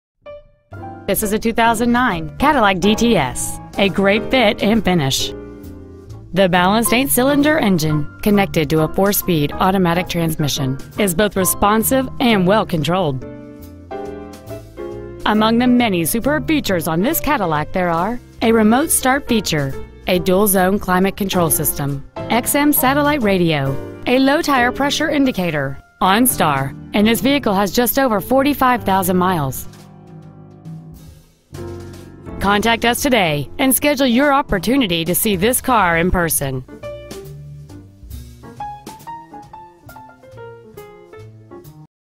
2009 Cadillac DTS Conyers GA
We've been honored to serve the Conyers GA area, we promise that your experience at our dealership will exceed your expectations!

 Year: 2009
 Make: Cadillac
 Model: DTS
 Engine: Gas V8 4.6L/279
 Trans.: Automatic
 Exterior: Black Raven
 Miles: 45,060
 Interior: Ebony

 http://

 2009 Cadillac DTS Conyers GA